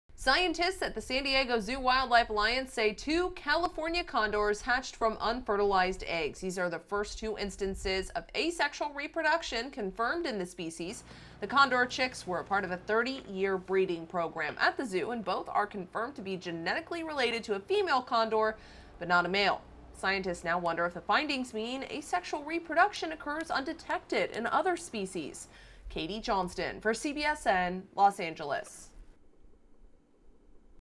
San Diego Zoo Wildlife Alliance Confirms 2 California Condor Chicks Were Hatched Asexually
Scientists at the San Diego Zoo Wildlife Alliance say two California condors have hatched from unfertilized eggs, the first two instances of asexual reproduction confirmed in the species. Katie Johnston reports.